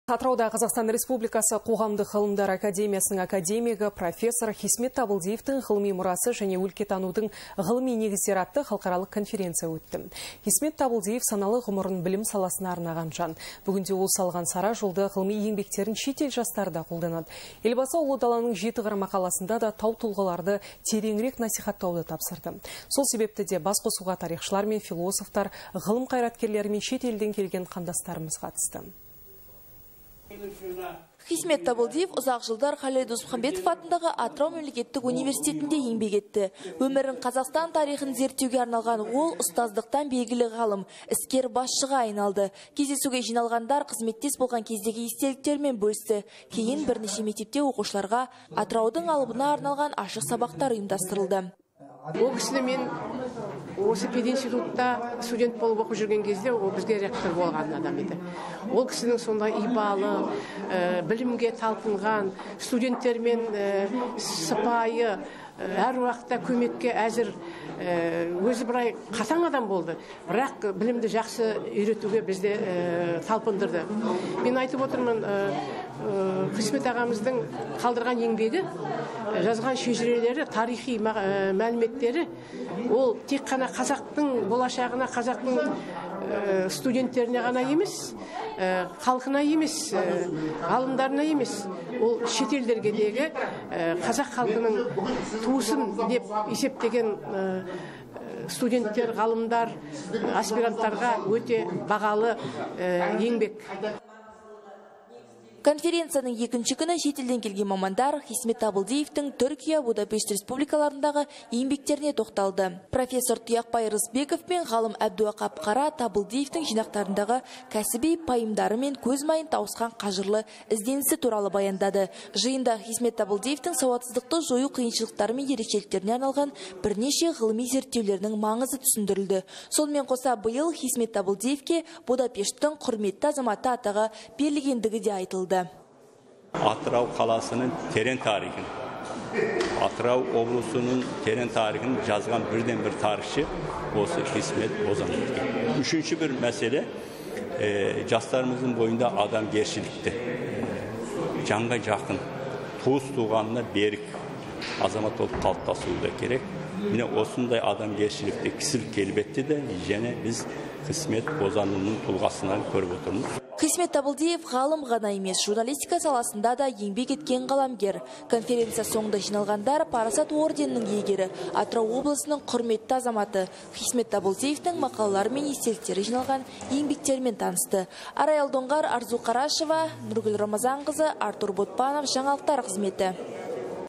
BELGİLİ GALIM QOGAM QAYRATKERİ PROF. HİSMET TABILDIYEV
26 qantar küni Atırav qalasında qatısıp qaytqan belgili galım jane qogam qayratkeri Prof. Hismet Tabıldiyev tuvralı qalıqaralıq konferentsiyaga qatıstı Atırav telediydarının qabarı. Tüp nusqa üşin qaranız